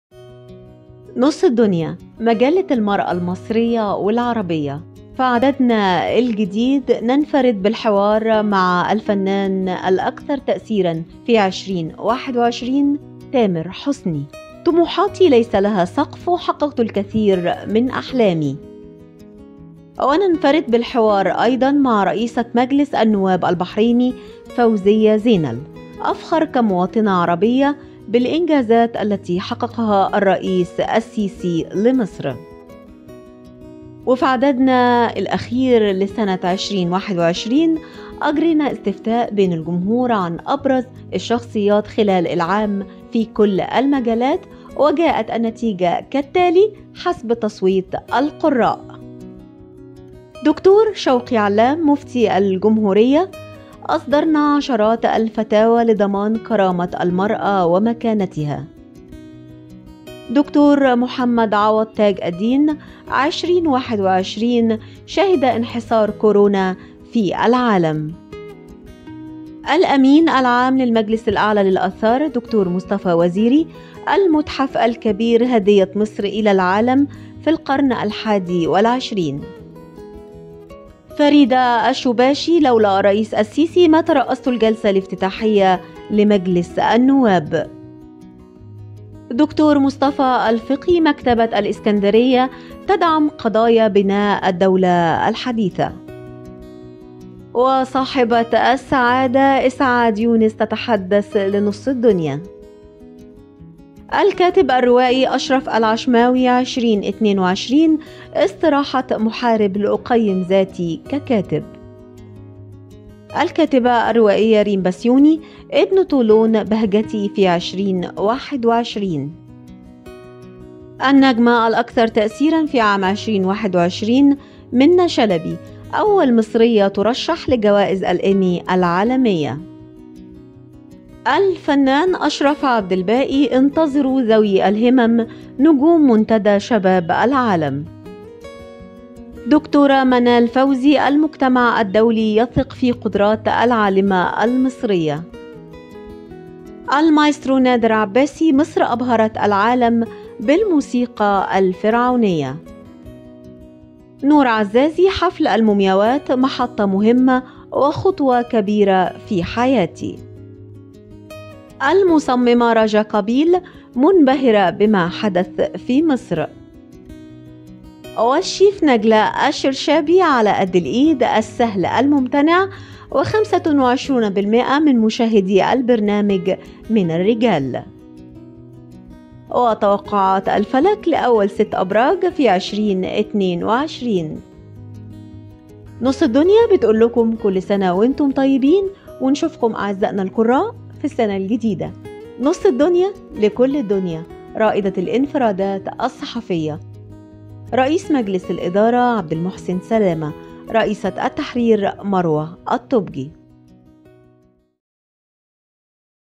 أبرز موضوعات العدد 1664 من مجلة نصف الدنيا
كل عام وأنتم بخير .. عام سعيد على كل متابعينا في العدد الجديد من نصف_الدنيا مجلة المرأة المصرية والعربية و رائدة الانفرادات الصحفية
- ننفرد بالحوار مع الفنان الأكثر تأثيرًا في عام 2021 تامر_حسنى: طموحاتي ليس لها سقف وحققت الكثير من أحلامي
- وانفردنا أيضًا بالحوار مع رئيسة مجلس النواب البحريني فوزية_زينل: أفخر كمواطنة عربية بالإنجازات التي حققها الرئيس السيسي لمصر
وفي استطلاع الشخصيات الأكثر تأثيرًا والأبرز لعام 2021 الذي أجرته نصف الدنيا اختار القراء الأفضل  في كل مجال وقمنا بعمل حوارات معهم أبرزهم:
- د. شوقي علام مفتي_الجمهورية :أصدرنا عشرات الفتاوى لضمان كرامة المرأة ومكانتها
- د.محمد عوض تاج الدين مستشار رئيس الجمهورية: أحزنني فقد رفيقة الكفاح والدرب خلال عام 2021
- فريدة_الشوباشي: لولا الرئيس السيسي ما ترأستُ الجلسة الافتتاحية ل
- الأمين العام للمجلس الأعلى للآثار د. مصطفى_وزيري : المتحف الكبير هدية مصر إلى العالم في القرن الحادى والعشرين
- د.مصطفى الفقي
- أشرف عبد الباقي
- منة_شلبى.. أول مصرية تُرشَّح لجوائز الإيمي العالمية
- الكاتب الروائى أشرف العشماوى: ٢٠٢٢ «استراحة_محارب» لأقيِّم ذاتى ككاتب
- حصلت على ثلاث جوائز المُصمِّمة راجا_قابيل: منبهرة بما حدث في مصر
تجدونه بالأسواق غدًا صباح يوم الخميس 30 ديسمبر .. بادروا بحجز نسختكم من الآن 😉